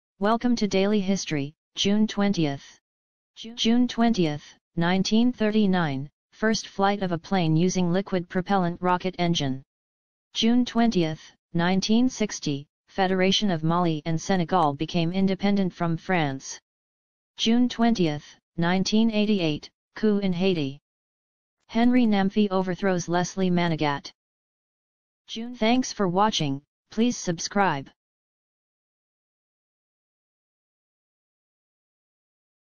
#3 June 20 in history
daily history, June 20.

June 20, 1782: US Congress approved Great Seal of the United States of America.

June 20, 1866: Italy declared war on Austria thus beginning the Third Italian War of Independence.

June 20, 1939: First flight of a plane using liquid propellant rocket engine.

June 20, 1960: Federation of Mali and Senegal became independent from France.

June 20, 1988: Coup in Haiti, Henri Namphy overthrows Leslie Manigat.

June 20,  1992: constitution of
Paraguay was formed.

June 20, 2018: Algeria turned off its internet to stop school students cheating during exams.